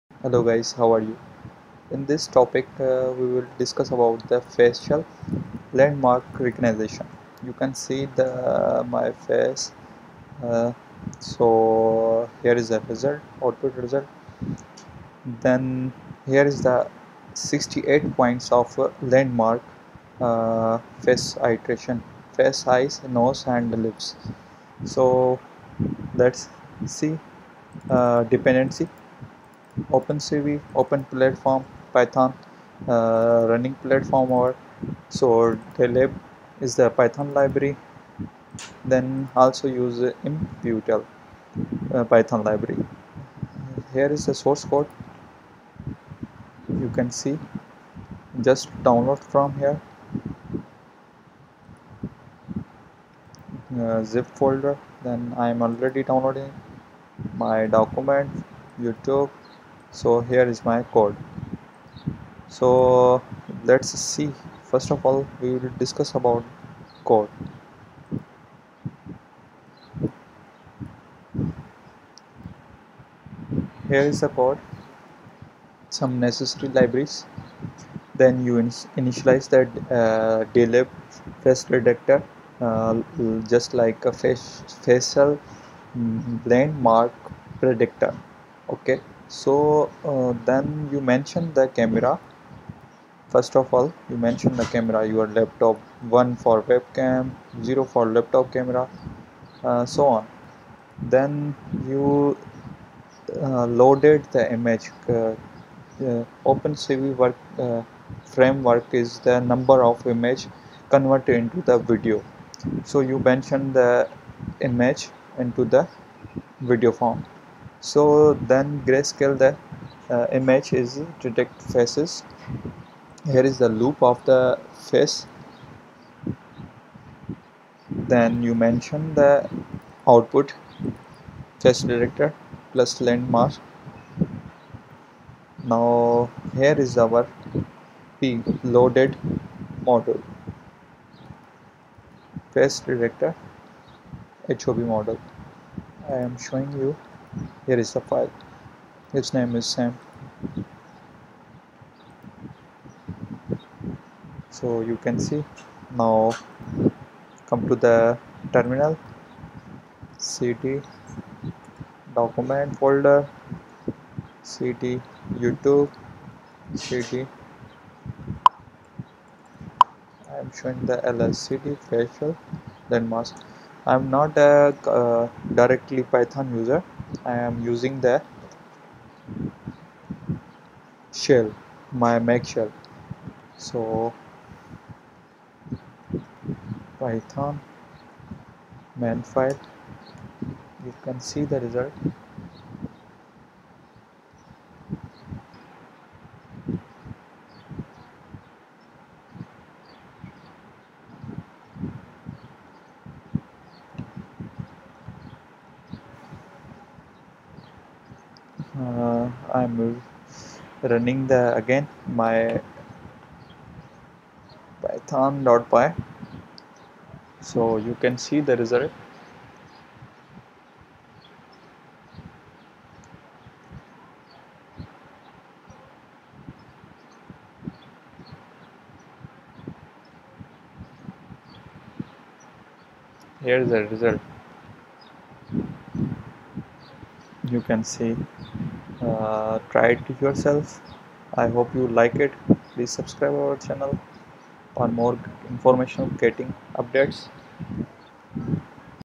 Face Landmark Detection using dlib - Python OpenCV
Facial landmark detection is detecting landmarks or regions of interest (key points) on the face like Eyebrows, Eyes, Nose, Mouth, and Jaw silhouette.

Some applications of facial landmark detection are face swap, head pose detection, detecting facial gestures, gaze direction, etc.

OpenCV stands for Open Source Computer Vision Library, to read and process our image.